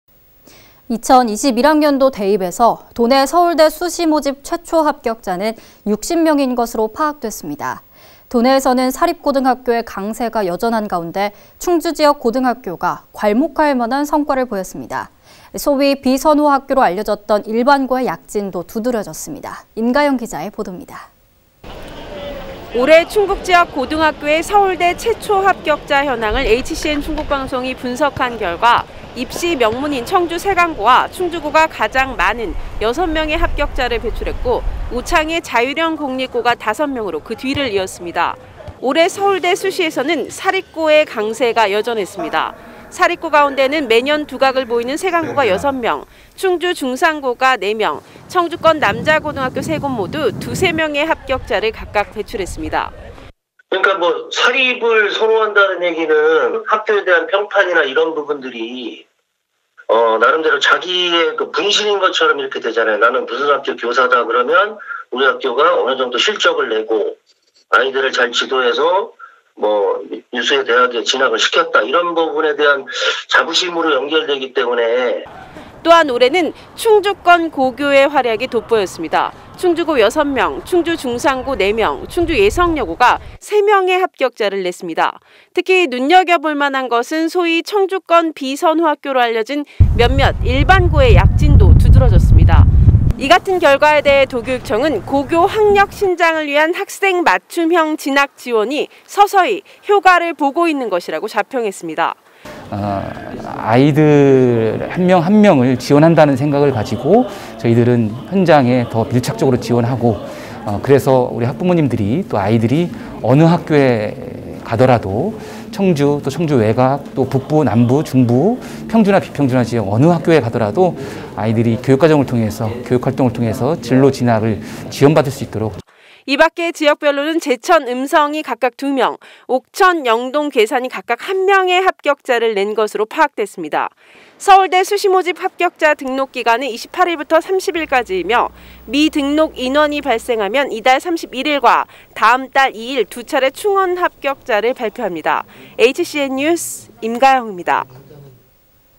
서울대 수시 사립고 강세 여전...충주권 고교 '도약'/충북 현대HCN
2021학년도 대입에서 도내 서울대 수시모집 최초 합격자는
60명인 것으로 파악됐습니다.

도내에서는 사립고등학교의 강세가 여전한 가운데
충주지역 고등학교가 괄목한 만할 성과를 보였습니다.

소위 비선호 학교로 알려졌던 일반고의 약진도
두드러졌습니다.

임가영 기자의 보돕니다.